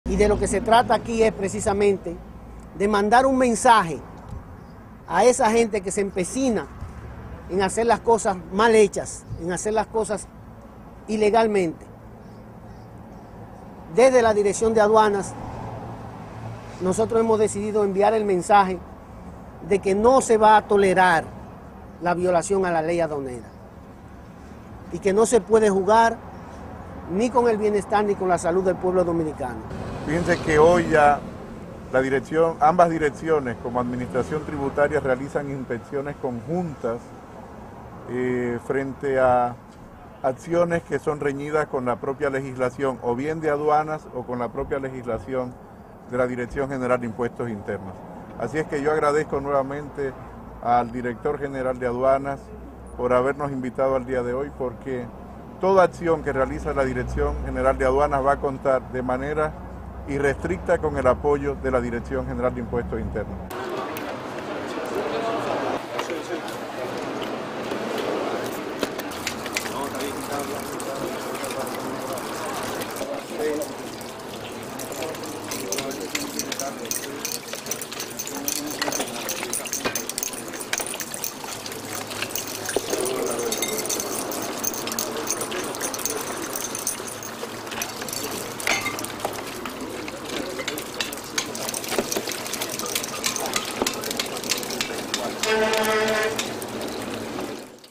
DGA destruye 800 cajas de bebidas alcohólicas
La Dirección General de Aduanas (DGA) en un claro mensaje de combate al tráfico ilícito de mercancías destruyó ayer unas 800 cajas de bebidas alcohólicas incautadas por entrar de contrabando al país. El lote tenía un valor de RD$7.5 millones.